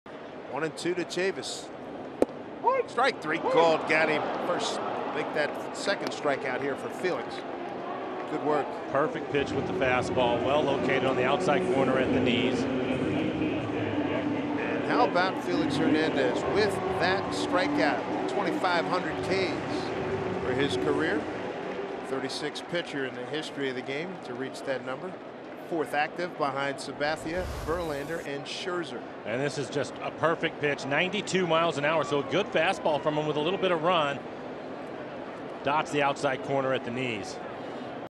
Félix Hernández notches 2,500th career strikeout | May 11, 2019 @ Red Sox
5/11/19: Félix Hernández strikes out Michael Chavis at Fenway Park to collect his 2,500th big league strikeout, becoming just the 36th MLB pitcher to reach milestone